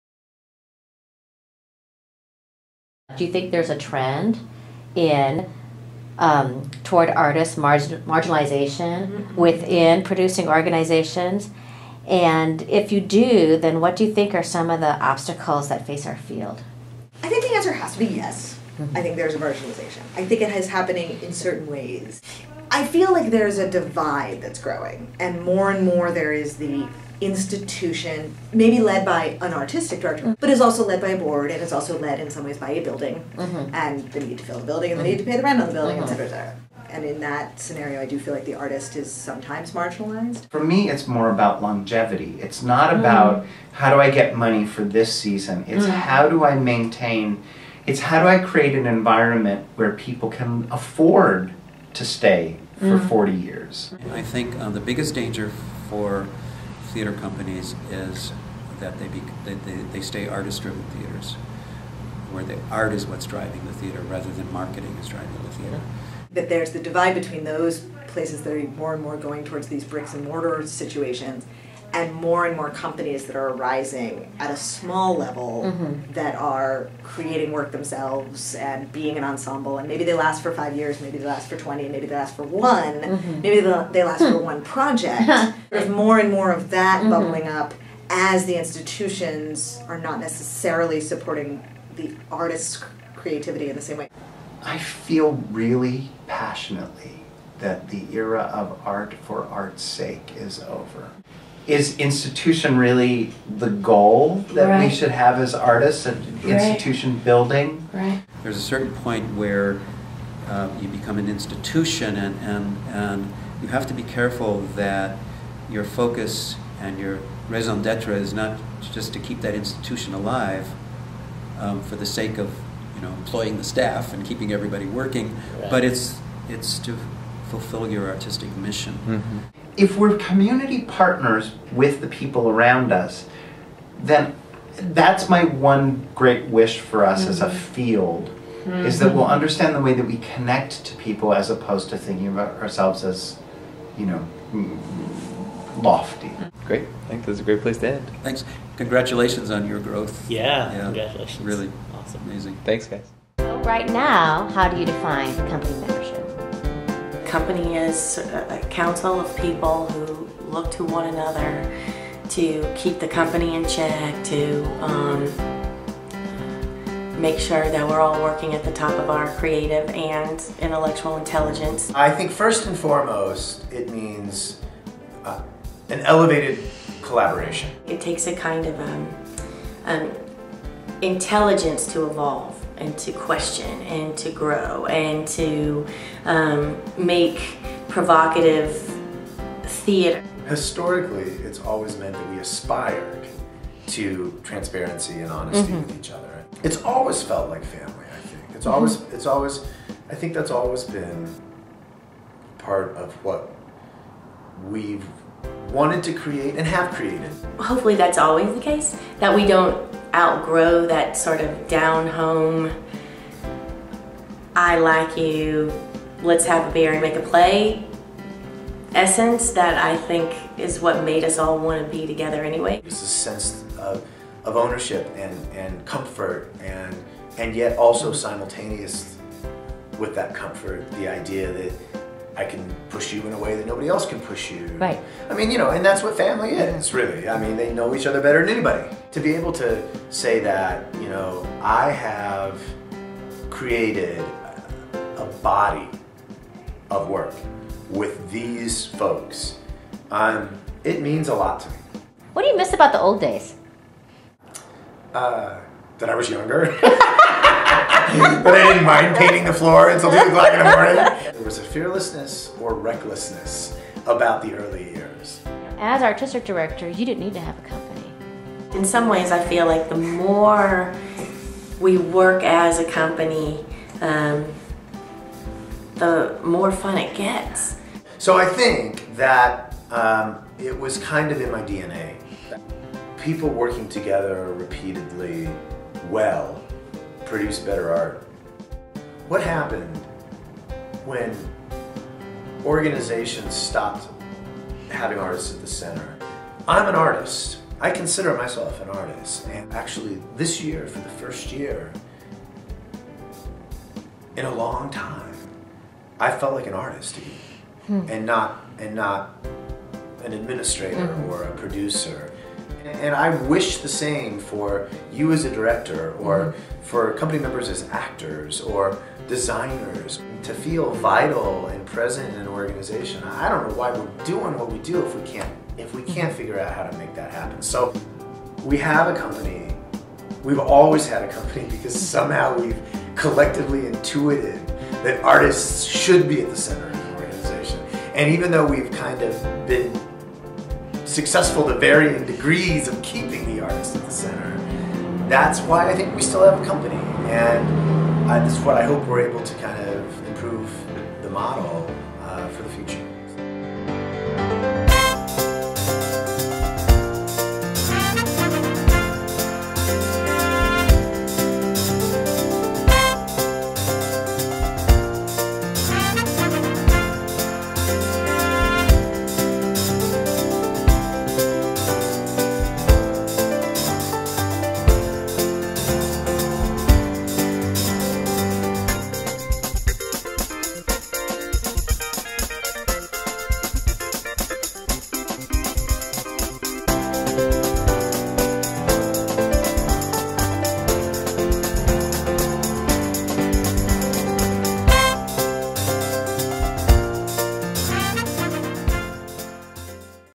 Curious A-Ha! Video Part 2
The Curious Theatre A-Ha! Core Team visited theatres across the country armed with the question "Why Company?" This video includes insightful comments from a sampling of our interviews with Artistic and Managing Directors of some of the most innovative and successful theatre in America.  This is Part 2